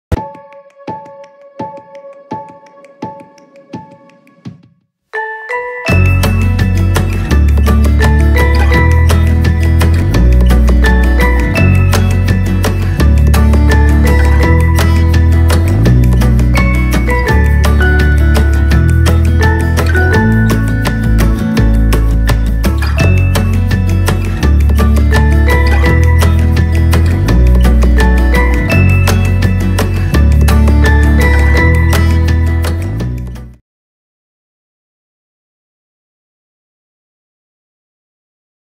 Rosieglitterqueen Lol fashion crush opening stop motion fun!
Fashion crush opening themselves! stop motion fun for a quick opening of these awesome outfits. Got a duplicate but I love them anyway.